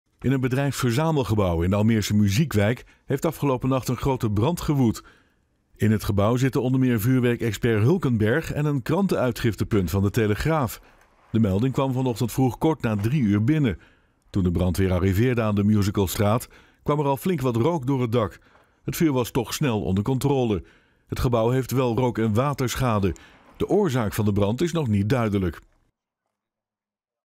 Grote brand op bedrijventerrein Muziekwijk in Almere
In een bedrijfsverzamelgebouw in de Almeerse Muziekwijk heeft een grote brand gewoed. De melding kwam kort na drie uur binnen in de nacht van zaterdag op zondag.

Toen de brandweer arriveerde aan de Musicalstraat, kwam er al flink wat rook door het dak. Het vuur was toch snel onder controle. Het gebouw heeft wel rook- en waterschade. De oorzaak van de brand is nog niet duidelijk. In het gebouw zitten onder meer vuurwerkexpert Hulkenberg en een krantenuitgiftepunt van De Telegraaf.

De pers werd op een flinke afstand gehouden, volgens de woordvoerder van de brandweer had dat te maken met de forse rookontwikkeling en de mogelijke aanwezigheid van vuurwerk in het bedrijfsverzamelgebouw.